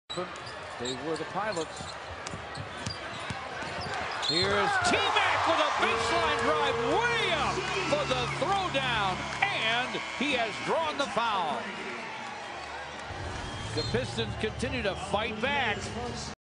Tracy McGrady Two Hand Monsterdunk vs. Memphis 13.1.2011
T-Mac drives on the basline and throws it down with power!!!
Detroit vs. Memphis  January 13th 2011